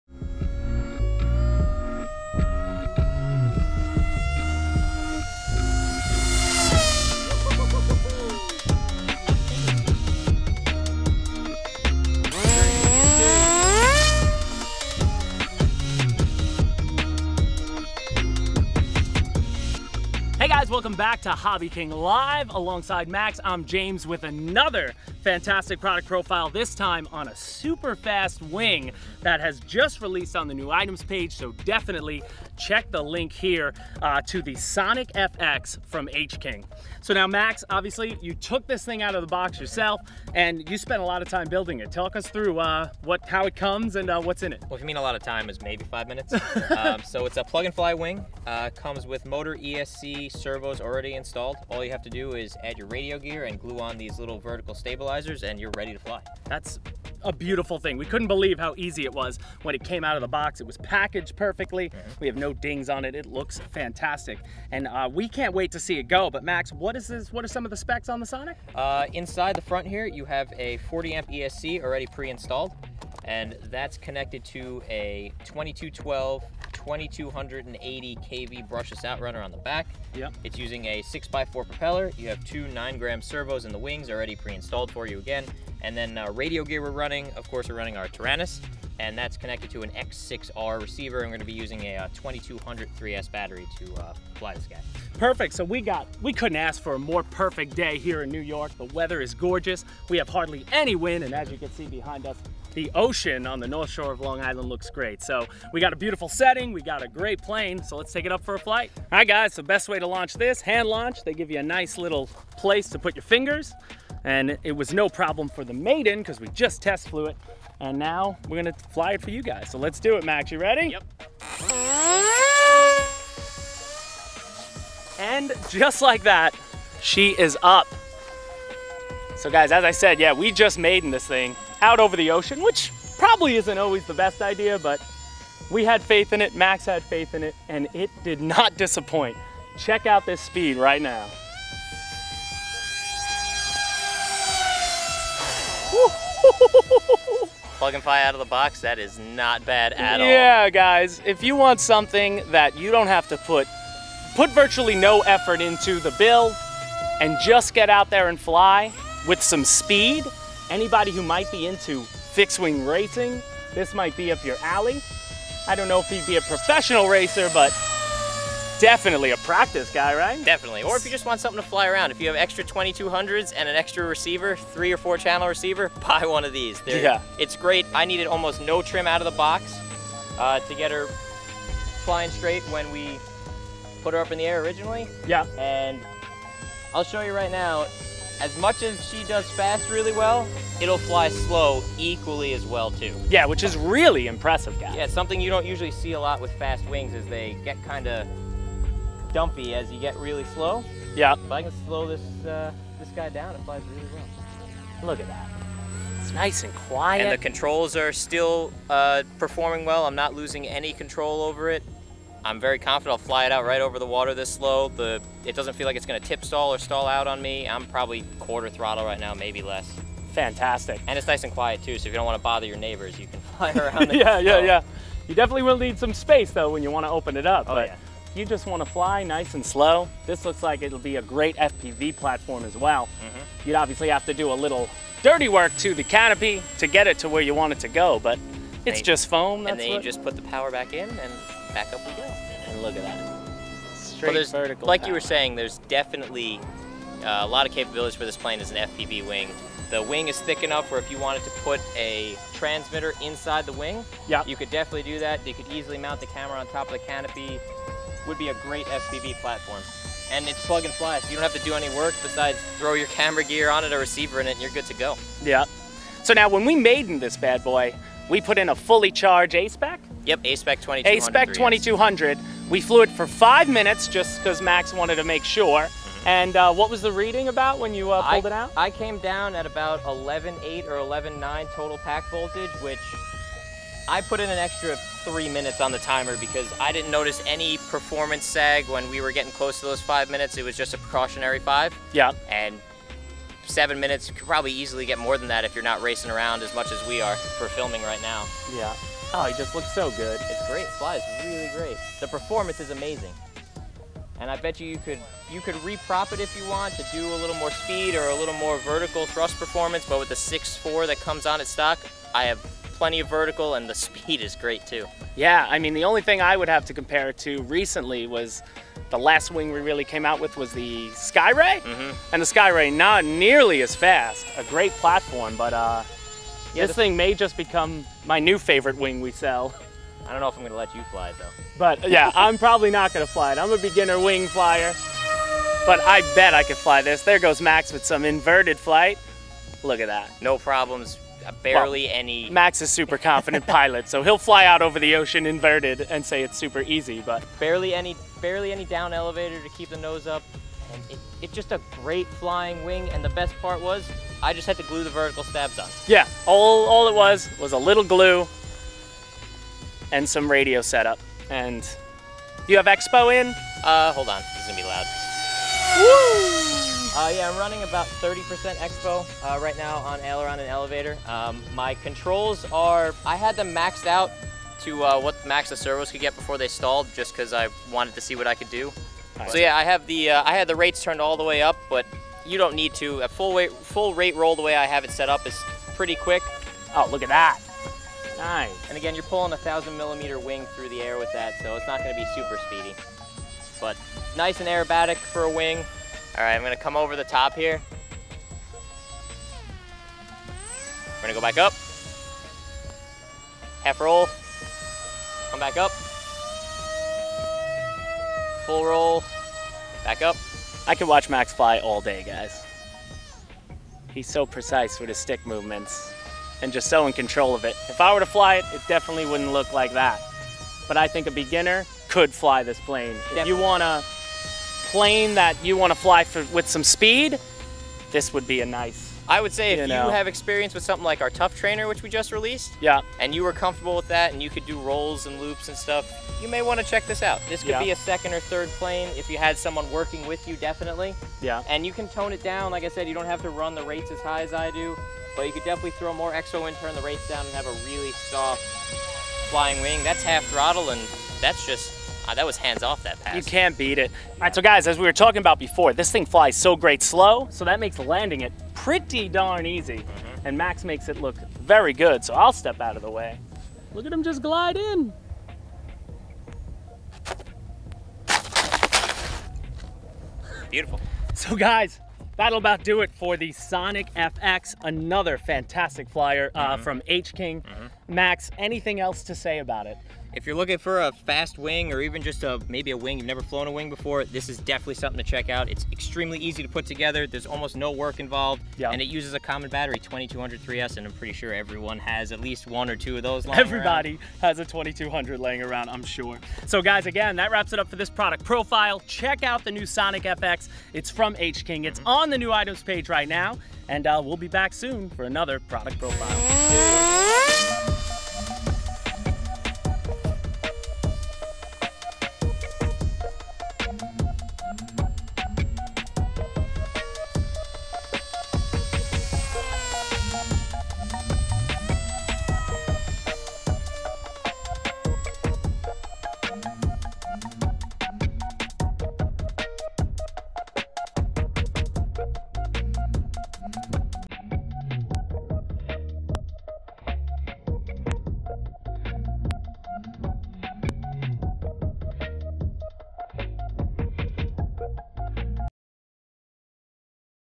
Sonic FX 1000mm EPO Wing - Product Profile - HobbyKing Live
James and Max are here to show you an unboxing and flight with the new Sonic FX Wing!